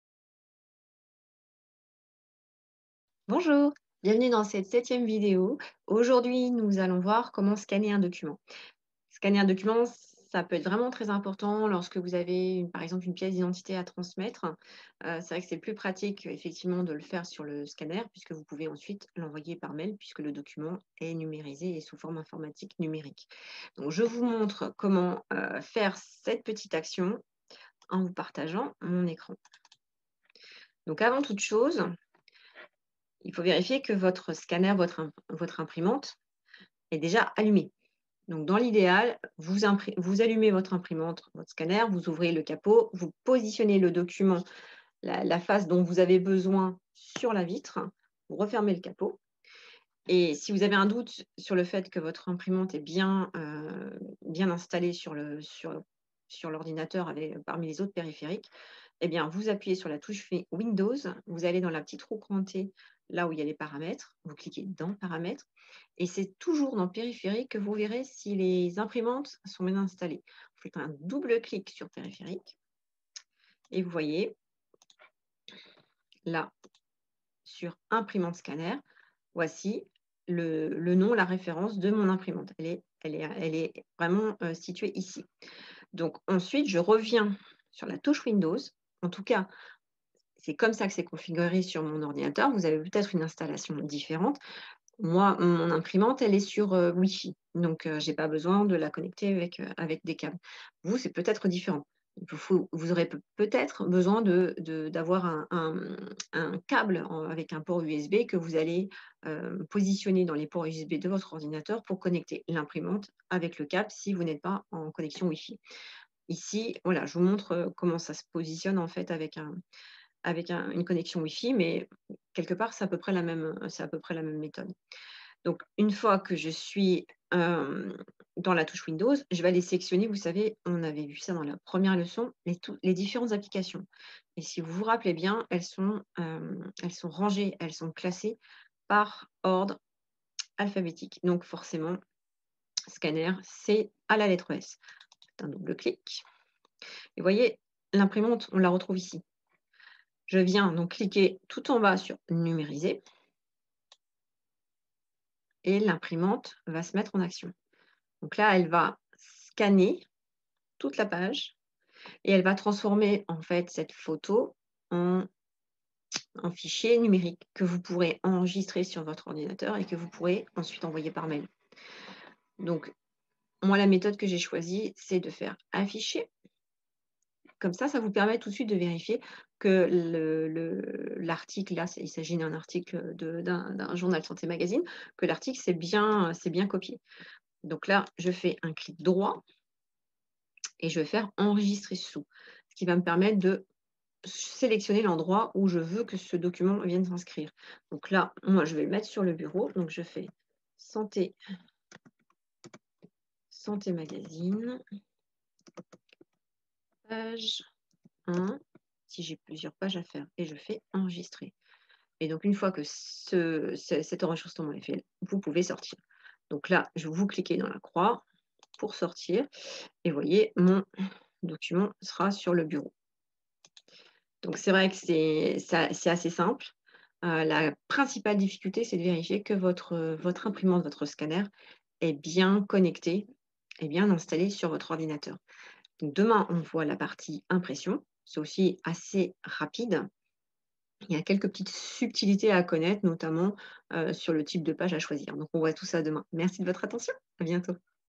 Comment Scanner un Document sur Windows 10
Dans cette vidéo, je vous guide à travers les étapes pour scanner un document facilement avec Windows 10, que vous utilisiez une connexion WiFi ou USB pour votre imprimante.

Suivez-moi pas à pas, depuis la vérification de la connexion de votre imprimante jusqu'à la numérisation et l'enregistrement de votre document.

Ce que vous apprendrez :

Vérification de l'Imprimante : Comment s'assurer que votre imprimante est correctement reconnue par votre ordinateur dans les paramètres.

Téléchargement de l'Application Scanner : Si vous ne trouvez pas l'application Scanner sur votre PC, je vous montrerai comment la télécharger facilement depuis le Microsoft Store. (Nota Bene : Pour ceux qui n'ont pas l'application Scanner installée, vous pouvez la télécharger gratuitement depuis le Microsoft Store.)

Utilisation de l'Application Scanner : Comment ouvrir et utiliser l'application pour numériser un document.

Positionnement du Document : L'importance de bien placer votre document sur la vitre du scanner pour obtenir la meilleure qualité possible.

Sauvegarde et Organisation : Comment enregistrer et renommer votre document numérisé, sélectionner l'emplacement de sauvegarde approprié, et s'assurer qu'il est bien rangé où vous le souhaitez.
Que vous ayez besoin de numériser des documents pour le travail, l'école ou vos affaires personnelles, cette vidéo vous fournira toutes les informations nécessaires pour le faire de manière efficace et organisée.